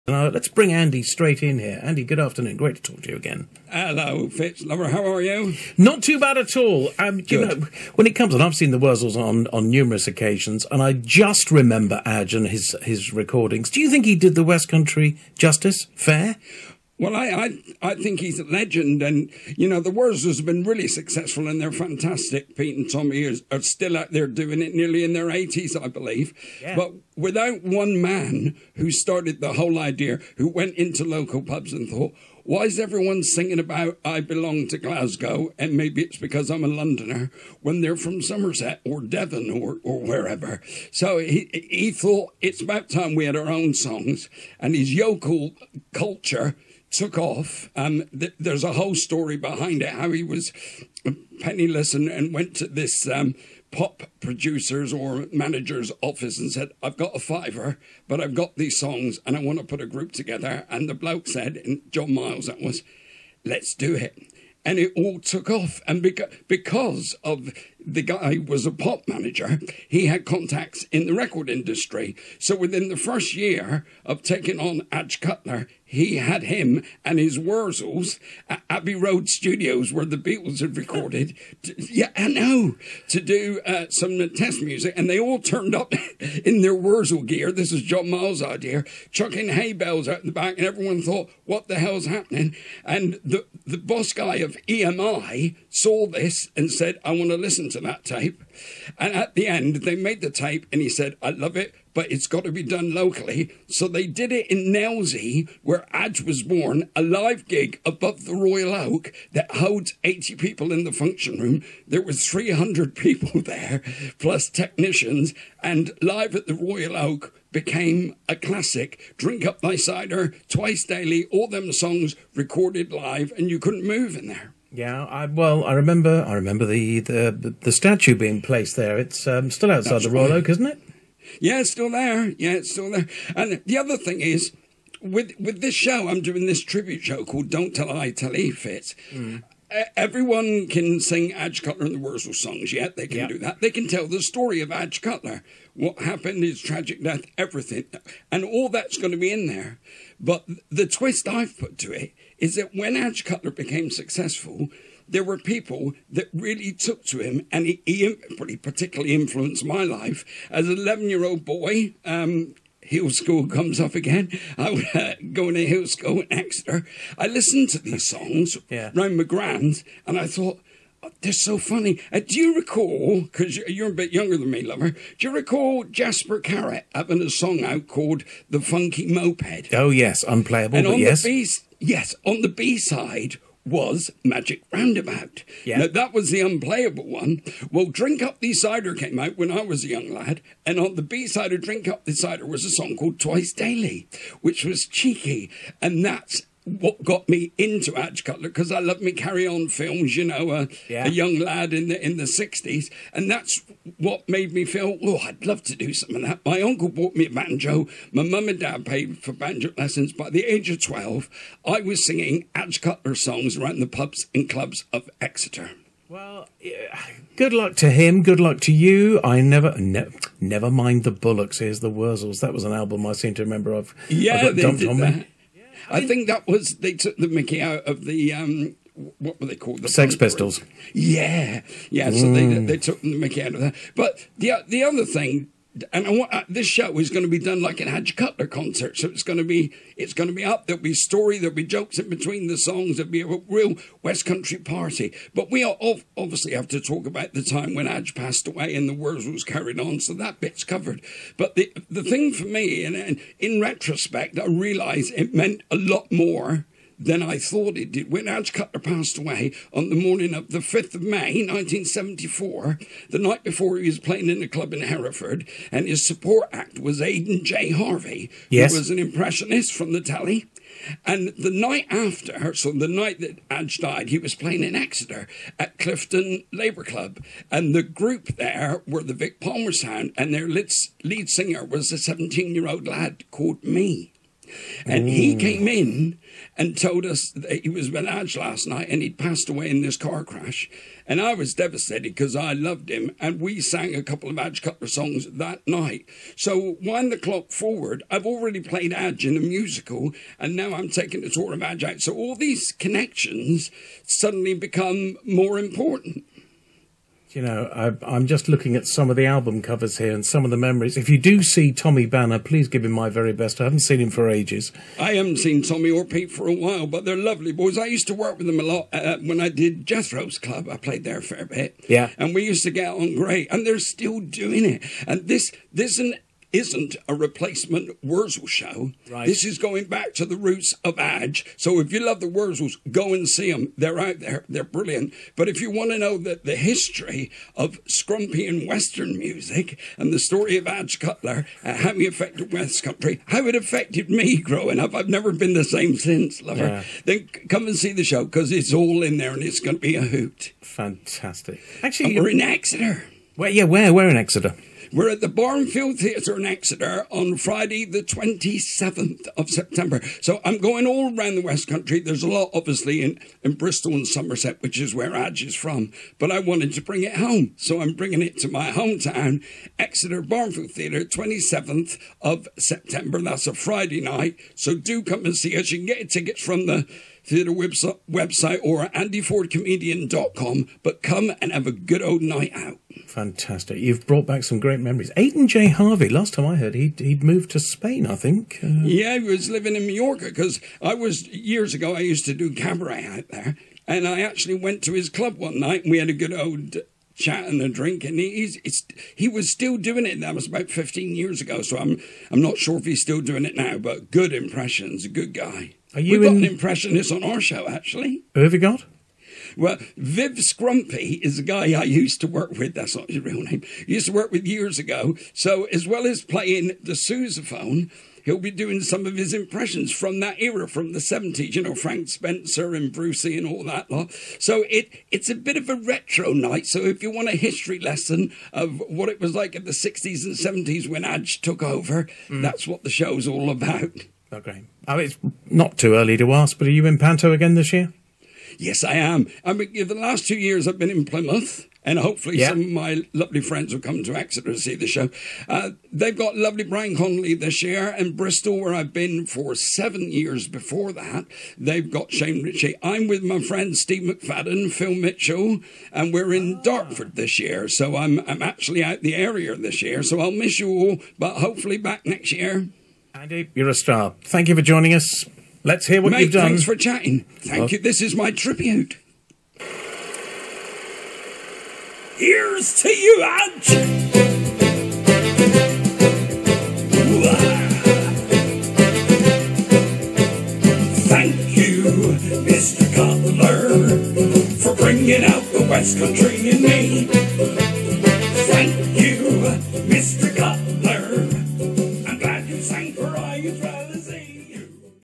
Andy Ford chats with David Fitzgerald on BBC Radio Devon
Andy's chat with BBC Radio Devon presenter about bringing his Adge Cutler tribute show to the Barnfield theatre in Exeter (Andy's home city) on september 27th